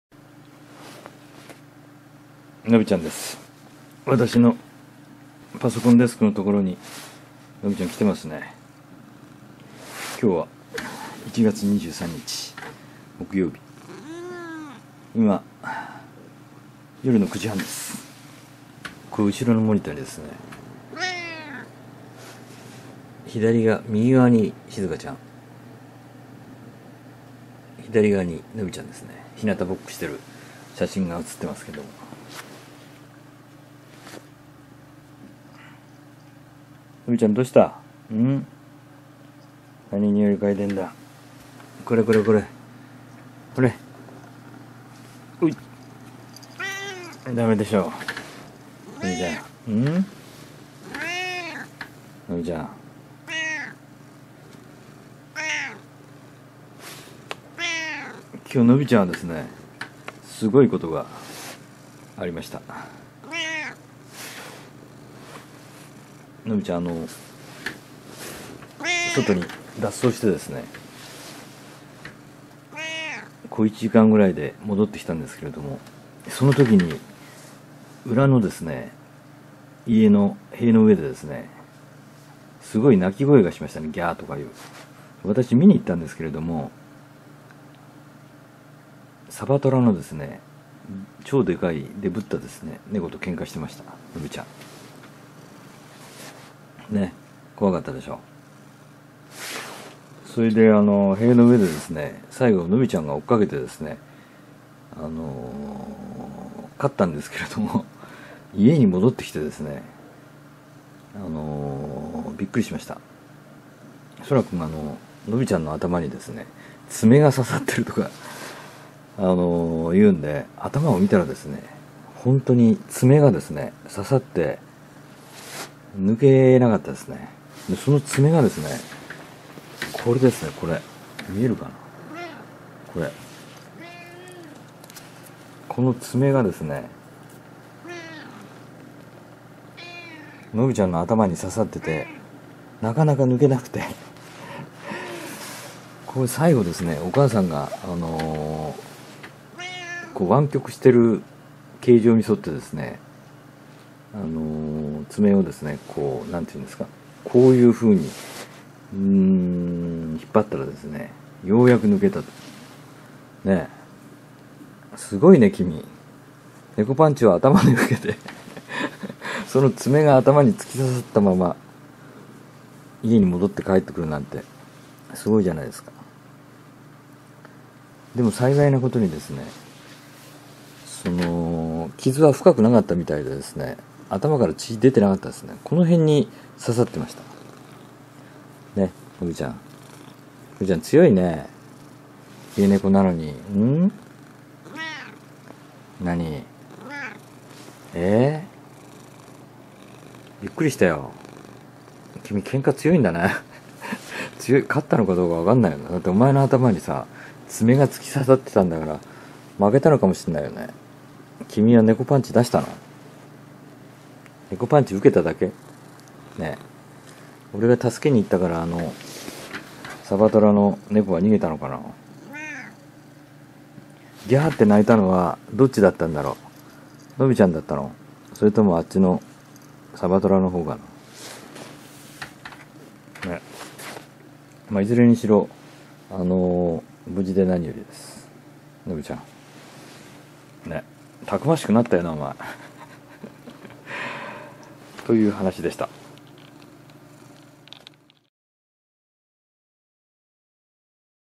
のび太クン ケンカに勝ったのか負けたのか 2020.1.23
ネコの鳴き声がするのでわが家の裏に見にいくと、のび太クンがよそのネコとケンカしていました。家の中にいると思っていたのびっくりしました。相手は太った大きなサバトラ君でした。のび太クンの名前を呼ぶと、サバトラは私にビビって逃げていきました。のび太クンは健気にも追いかけて行きましたが、しばらくして戻ってきました。びっくりしたのは、のび太クンの頭に爪が突き刺さっていたことです。ケンカ相手のサバトラ君のネコパンチをまともに頭に受けたようです。ソラ君と私が爪を抜こうとしましたが抜けませんでしたが、お母さんが抜いてくれました。それにしてものび太クン、家ネコで敵がいない安全な場所で育った坊ちゃんなのに、意外とたくましいので驚きました。

【なんて素晴らしい日々の動画再生リスト　旅や日曜大工やネコちゃんなど】